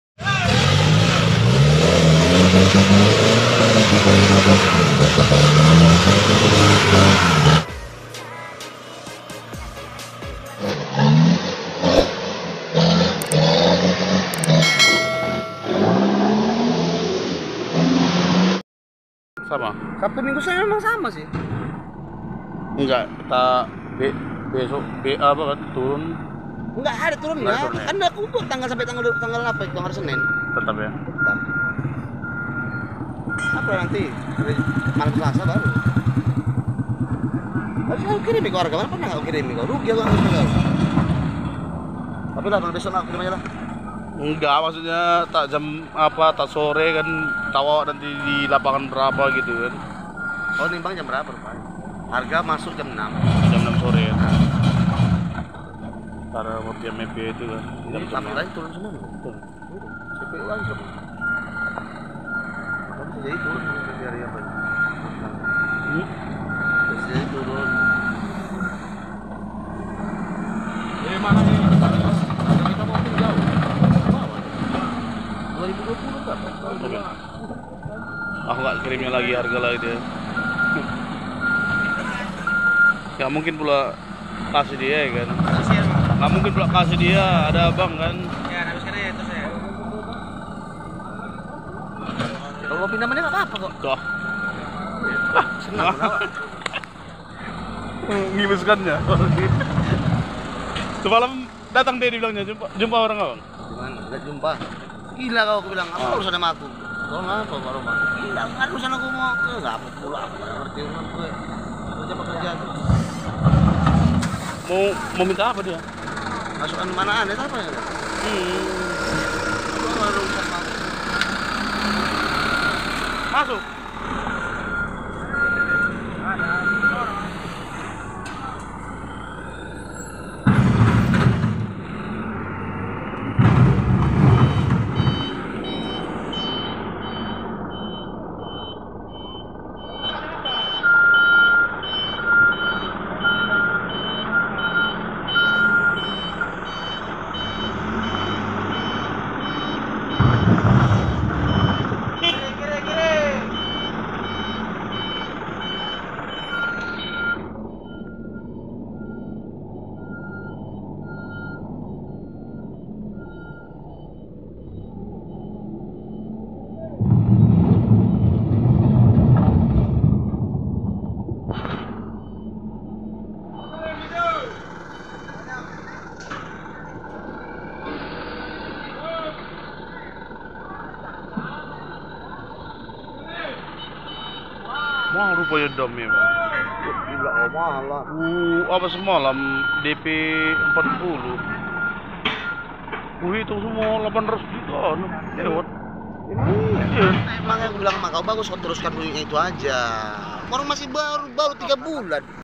si Oren bongkar muatan di pabrik
saat dam truk yang melintas jalan yang benar benar sangat berlumpur dan berlubang lubang